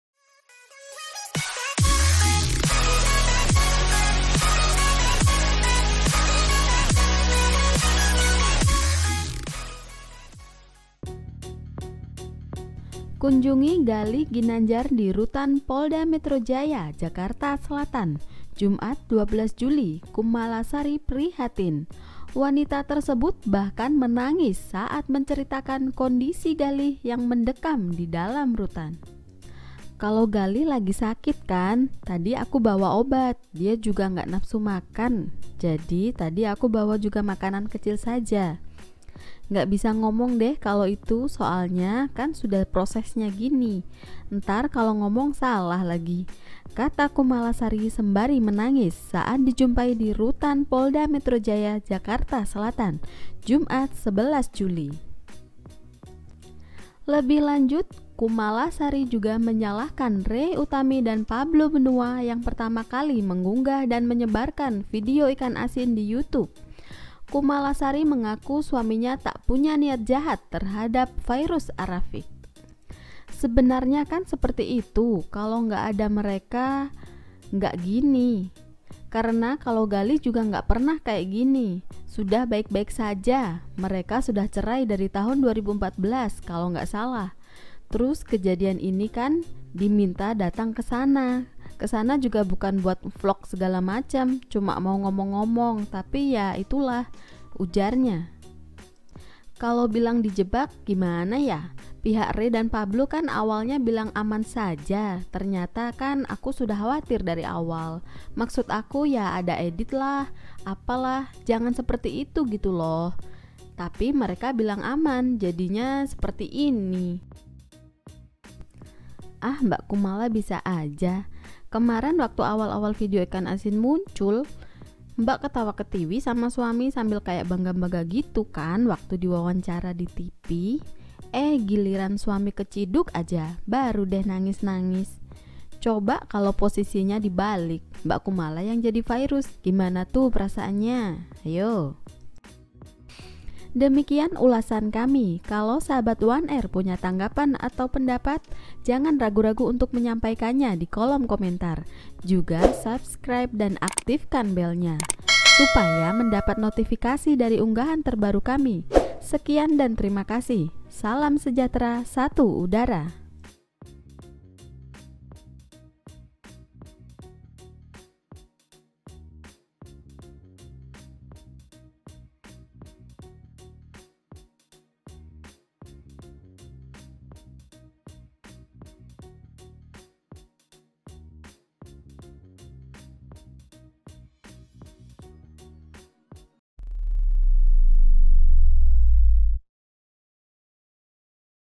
Salahkan Rey dan Pablo, Kumalasari Nangis Liat Kondisi Galih 𝒟𝒾𝓅𝑒𝓃𝒿𝒶𝓇𝒶
Channel ini menyajikan BERITA & OPINI❕ ,
Yang menggembirakan saja 😘👌,
Good Vibes ☮️☯️

Silahkan berkomentar dengan cerdas dan sopan 🙏🏻

Terima kasih 💋